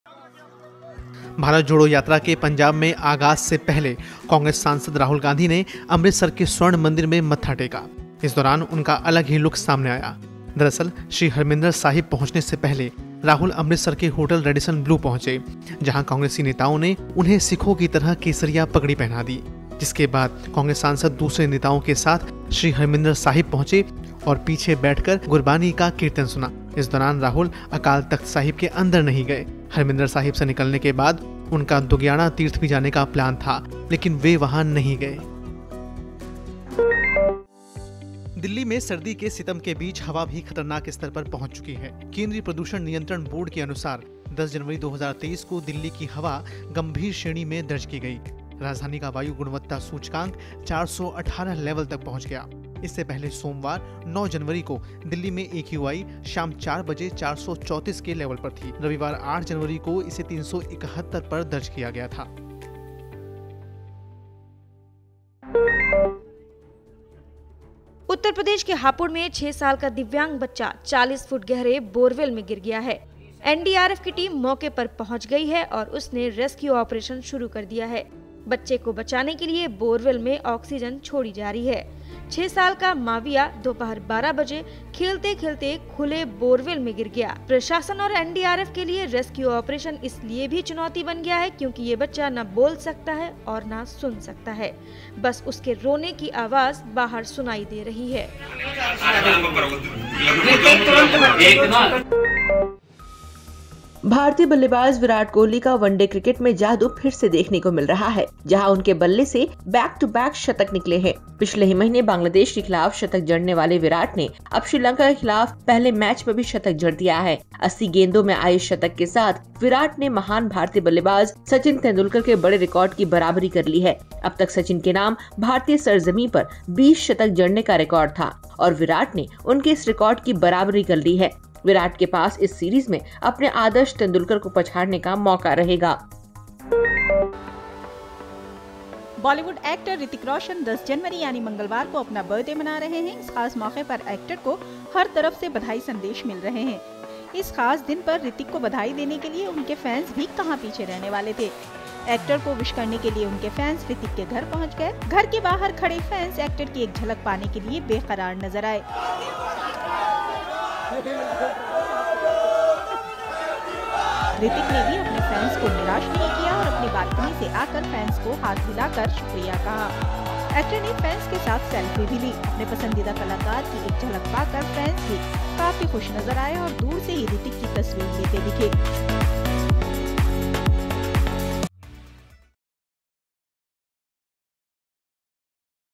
टॉप न्यूज़: केसरिया पगड़ी में हरमंदिर साहिब पहुंचे राहुल, दिल्ली में जहरीली हवा का अटैक
Bharat Jodo Yatra : केसरिया पगड़ी में हरमंदिर साहिब पहुंचे राहुल, दुर्ग्याणा तीर्थ का कार्यक्रम रद्द
Delhi Air Quality Index Update : दिल्ली में जहरीली हवा का अटैक. 400 पार AQI, अगले 3 दिन खतरनाक
Hapur Borewell: मासूम को बोरवेल ने सुरक्षित निकाला गया, NDRF ने 4 घंटे तक चलाया रेस्‍क्‍यू ऑपरेशन
वनडे में विराट कोहली के बल्ले से निकला लगातार दूसरा शतक, खतरे में सचिन तेंदुलकर का बड़ा रिकॉर्ड
Hrithik Roshan को बर्थडे विश करने उनके घर पहुंचे फैंस, एक्टर ने हाथ हिलाकर किया फैंस का अभिवादन

अब अपनी सुविधा के हिसाब से editorji ऐप पर खबरों को पढ़े, सुनें और देखें. अभी डाउनलोड करें